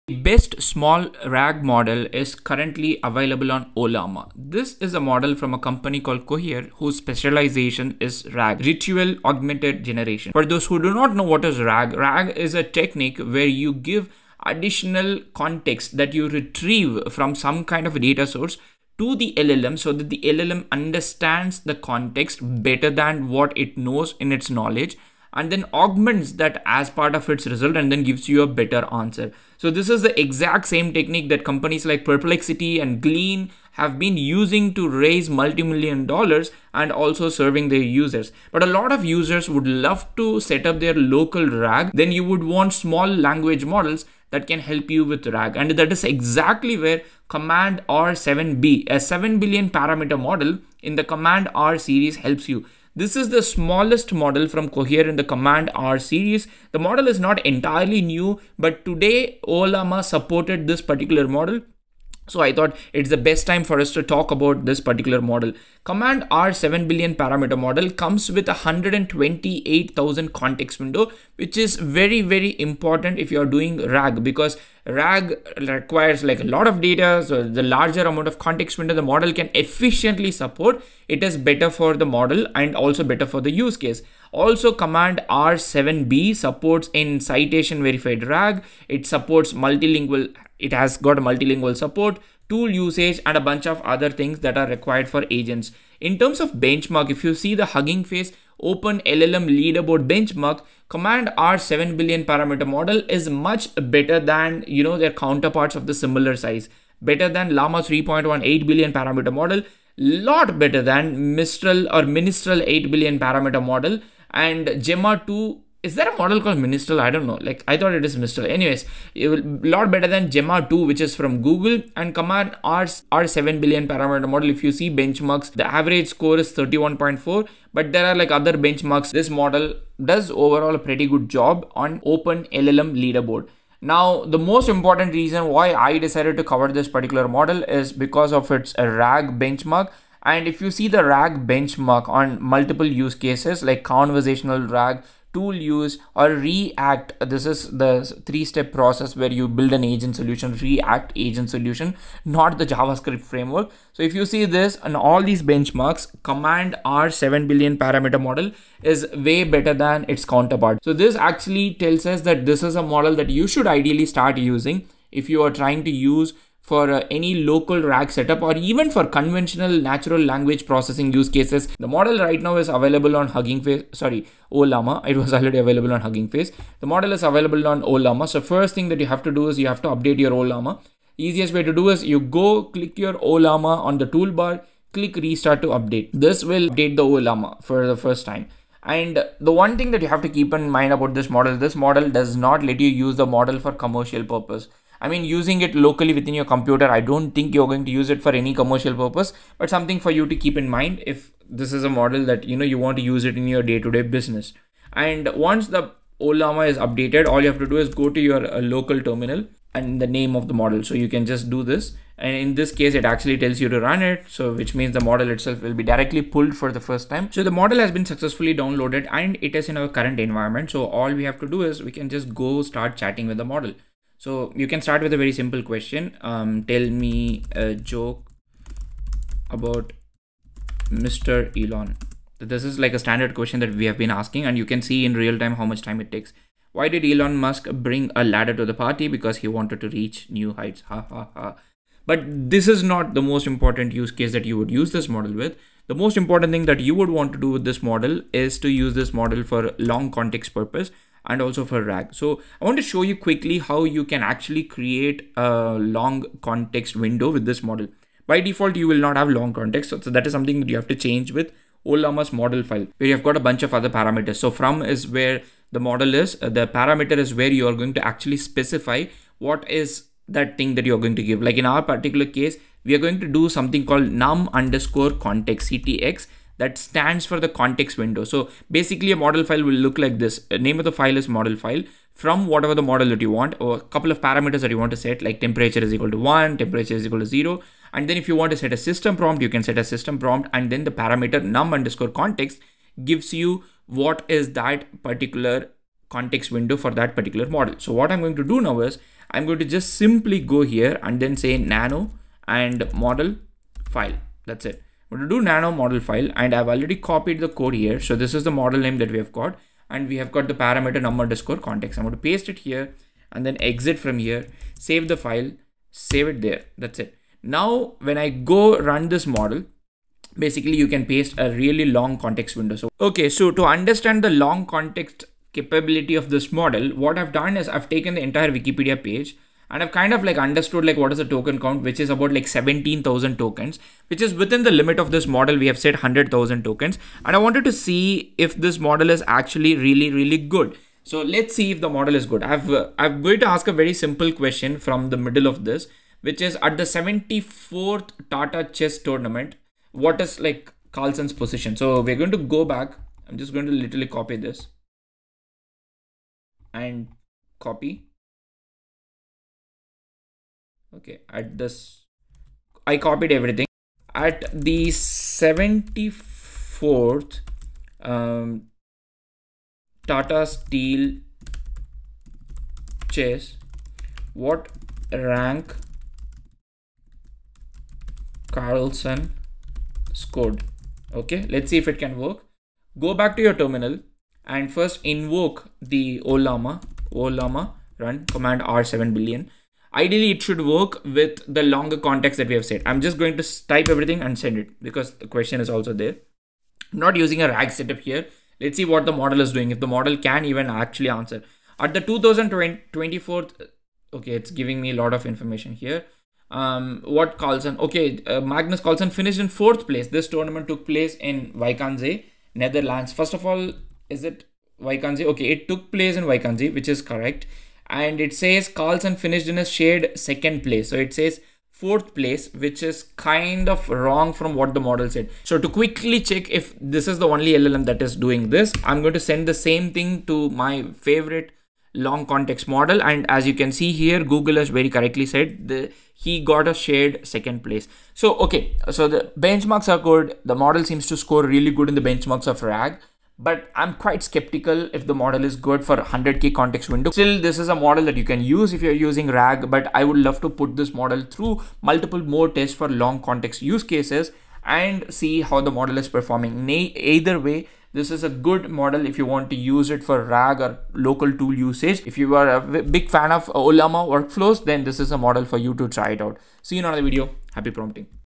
Command R7B - Small LLM for Local RAG!
The smallest model in Cohere's R series delivers top-tier speed, efficiency, and quality to build powerful AI applications on commodity GPUs and edge devices.

🔗 Links 🔗